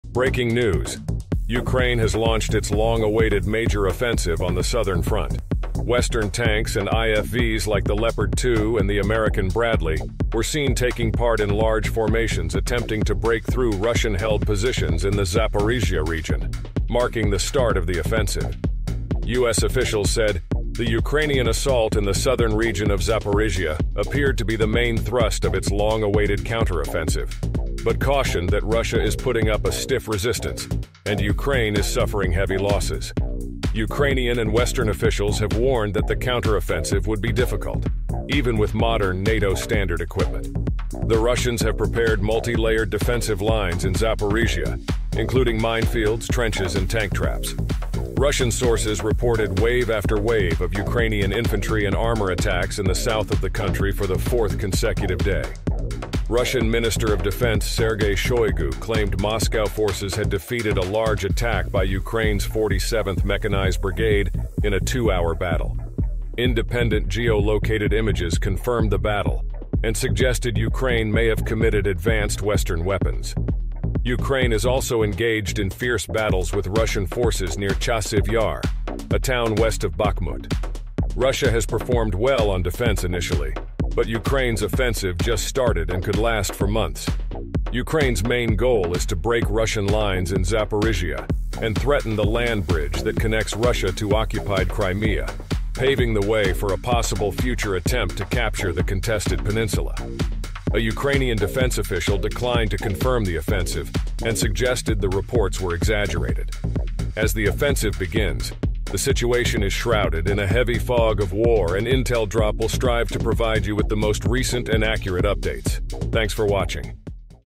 BREAKING: Ukraine launches massive offensive against Russia in the south!
Ukraine has finally started its long-awaited counterattack against Russian forces in the south of the country. Watch as Western tanks and IFVs like the Leopard 2 and the Bradley join the fight and try to break through Russian defenses in Zaporizhzhia. Will Ukraine succeed in its ambitious goal of threatening the land bridge to Crimea? Or will Russia crush the offensive and inflict heavy losses on Ukraine? Find out in this exclusive video from Intel Drop, your source for the latest updates on the war in Europe.